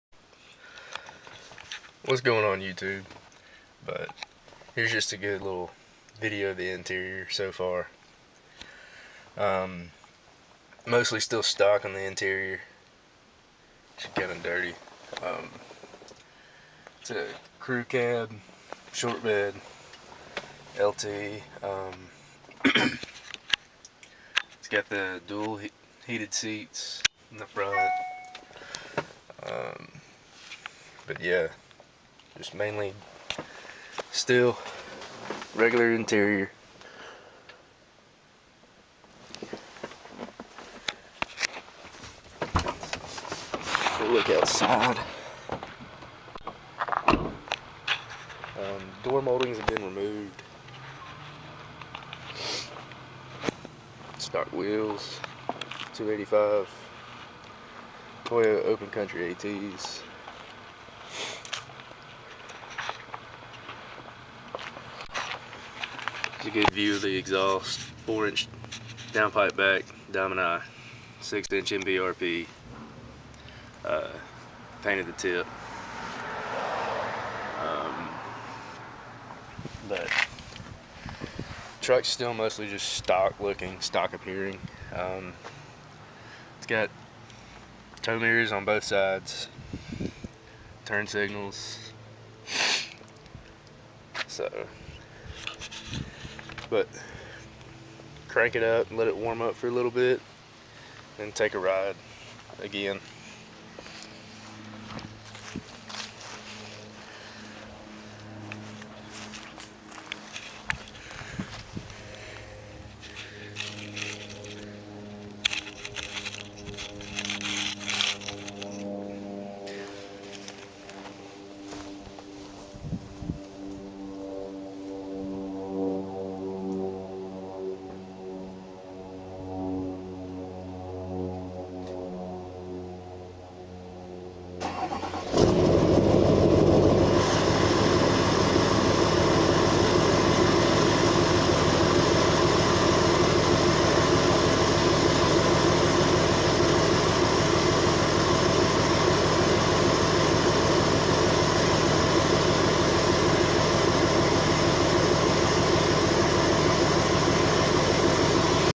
2006 Chevy 2500HD LBZ Duramax Exterior/Start up
View of the interior and exterior of my LBZ Duramax and start up